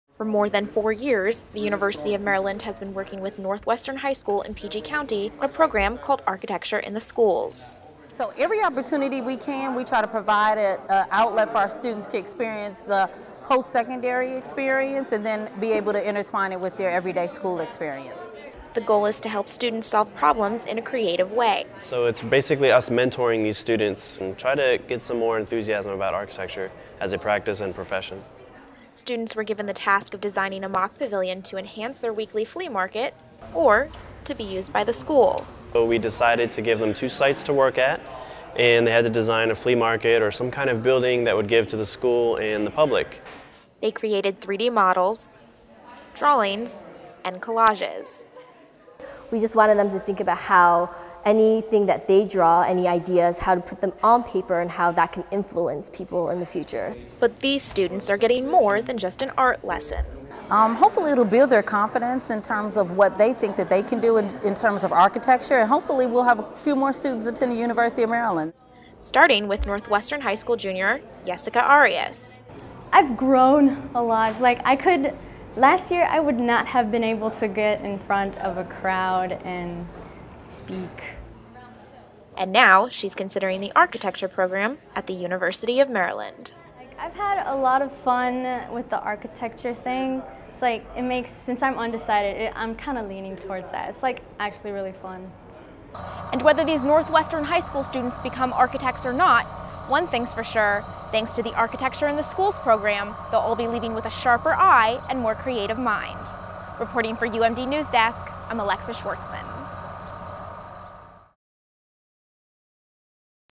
University of Maryland's Architecture in the Schools Program
Architecture in the Schools is a program that brings together students from the University of Maryland and Northwestern High School in Prince George's County (Maryland). Through an art class at the high school, students collaborate with Maryland grads and undergrads to complete tasks that will give them a new perspective into the world or architecture. Students from Northwestern were told to design a pavilion that could be placed in one of two designated sites at their school. They had the creative ability to choose the shape, look, and purpose of the structure.  The guidelines stated that the pavilion must function to enhance the school's weekly Saturday flea markets, or some aspect of school life for faculty and students. Although a pavilion was not actually built at the school, this project gave students the opportunity to think critically and get hands on experience in the architectural design process. Students arrived at the School of Architecture, Planning, and Preservation at the University of Maryland to present their final projects to a panel of judges. Story by Journalism Intern Alexa Schwartzman.